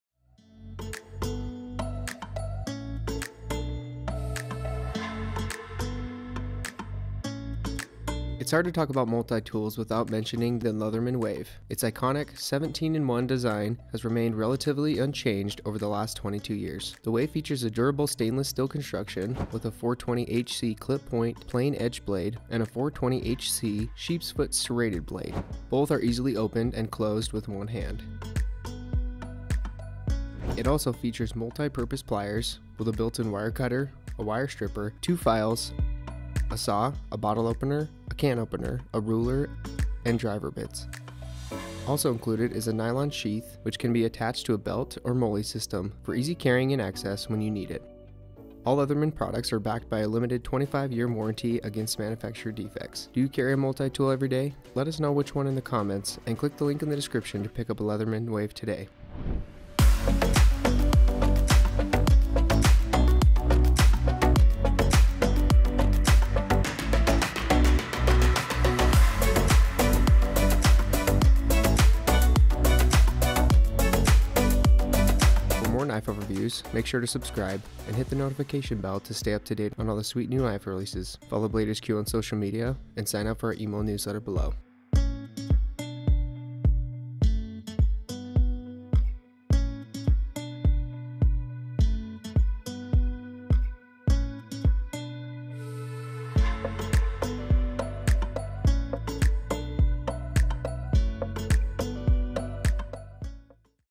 Leatherman Wave Plus | Knife Overview
It’s hard to talk about multi-tools without mentioning the Leatherman Wave. Its iconic, 17-in-1 design has remained relatively unchanged over the last 22 years.

The Wave features a durable stainless-steel construction with a 420HC clip point, plain-edge blade, and a 420HC sheepsfoot serrated blade. Both are easily opened and closed with one hand.

It also features multi-purpose pliers with a built-in wire cutter, a wire stripper, screwdrivers, two files, a saw, a bottle opener, a can opener, a ruler, and driver bits. Also included is a nylon sheath that can be attached to a belt or MOLLE system for easy carrying and access when you need it.

All Leatherman products are backed by a limited 25-year warranty against manufacturer defects.

Specifications:

Closed Length: 4"
Blade Length: 2.9"
Weight: 8.5 oz.
Locking Blade: Yes

Tools (17):

420HC Clip Point Knife
420HC Sheepsfoot Serrated Knife
Needlenose Pliers
Regular Pliers
Interchangeable Wire Cutters
Hard-wire Cutters
Wire Stripper
Large Screwdriver
Large Bit Driver
Small Bit Driver
Scissors
Wood/Metal File
Diamond-coated File
Saw
Bottle Opener
Can Opener
Ruler: 8 in | 19 cm
INCLUDED BITS: Phillips and Flat Tip Eyeglasses Screwdriver Bit, Phillips #1-2 and 3/16" Bit

0:00 Intro
0:8 Knife Overview
0:59 Full Specifications